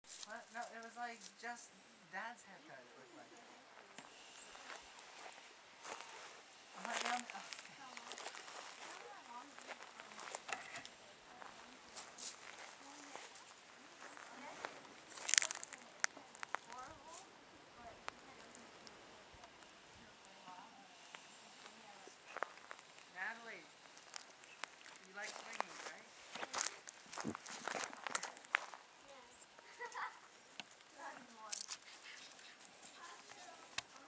Christmas 2011 (#11)
At the ranch in California Valley, CA USA.